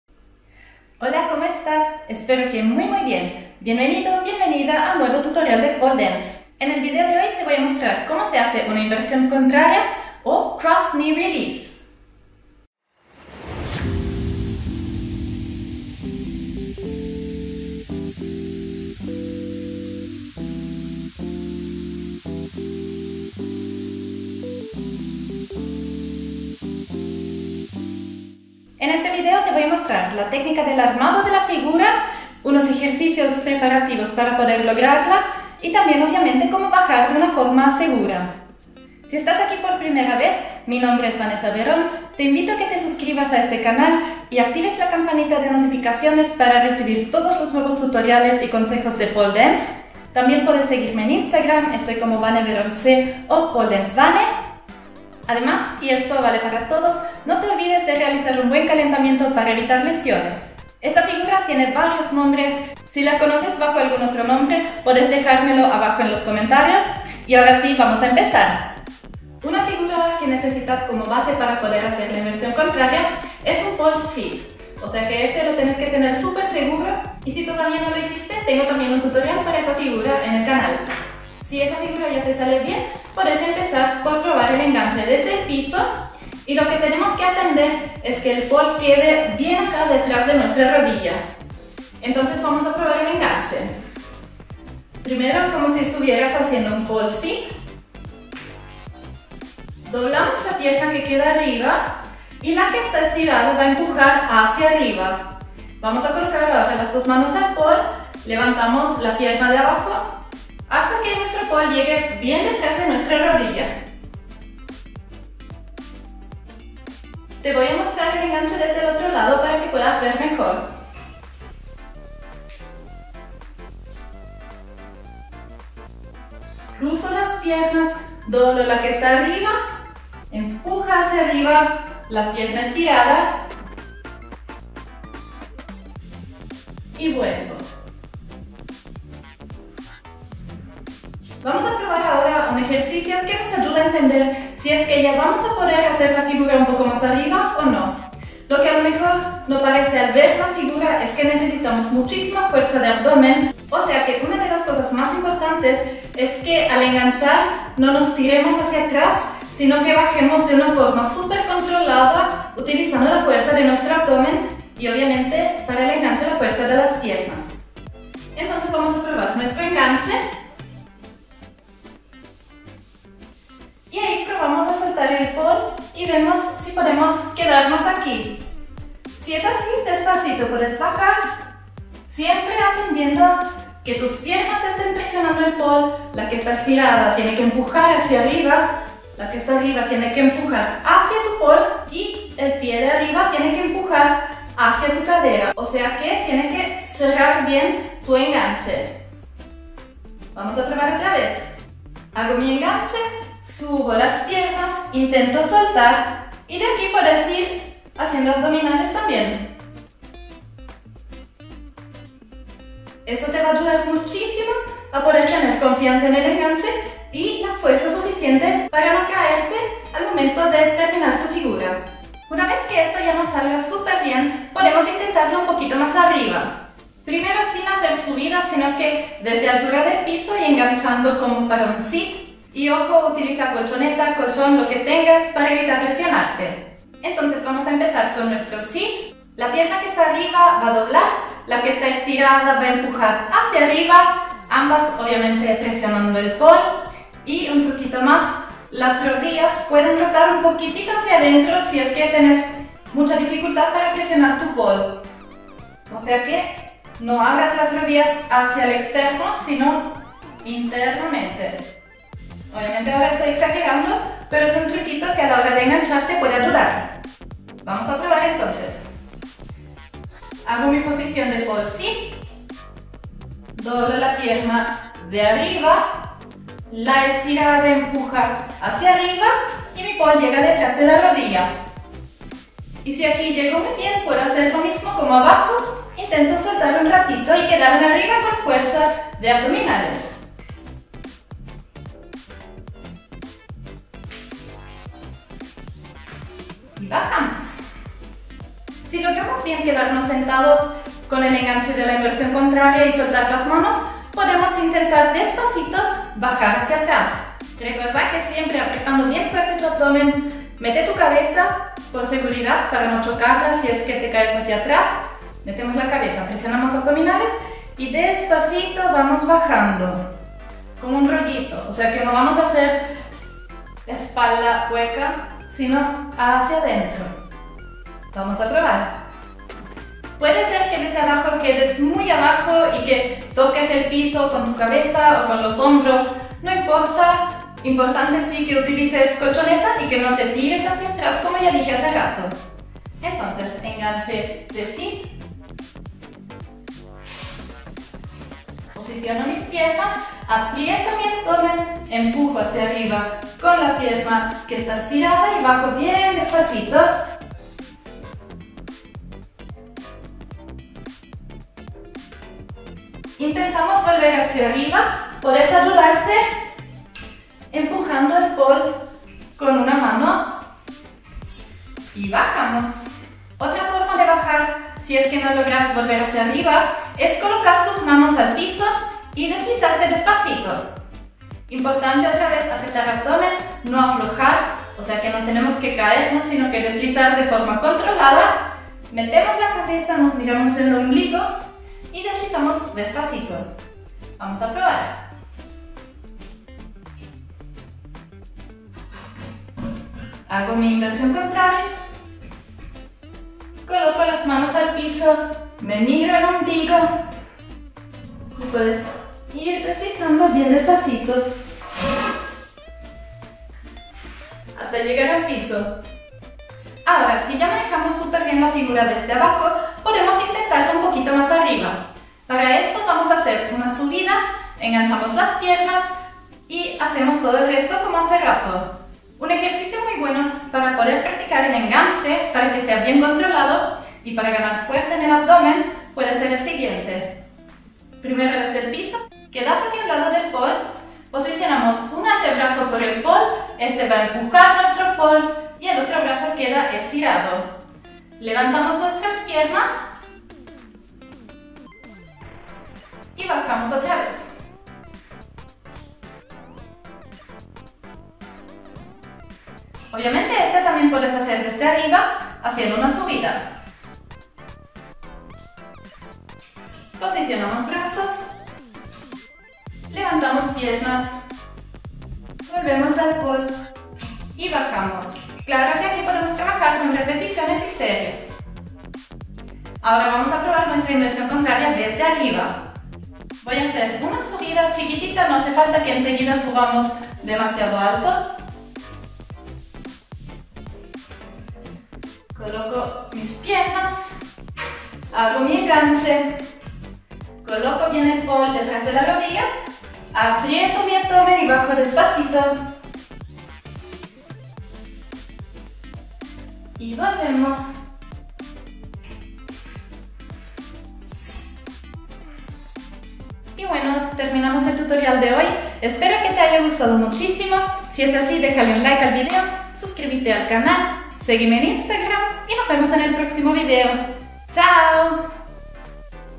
POLE DANCE tutorial: Inversión Contraria ~ Cross Knee release 🌟 (lay back, lean back, en español)
La inversión contraria muchas veces es considerada de nivel básico, pero en realidad es más complicadita de lo que parece! Necesitamos tener una buena base de fuerza y técnica para lograr el armado y poder sostenerla y salir de la figura sin caernos. En este video te muestro algunos ejercicios preparativos para poder lograr este truco paso a paso, te enseño el armado y algunas maneras de bajarnos del cross knee release. Conoces algún otro nombre para esta figura? Escribelo en los comentarios!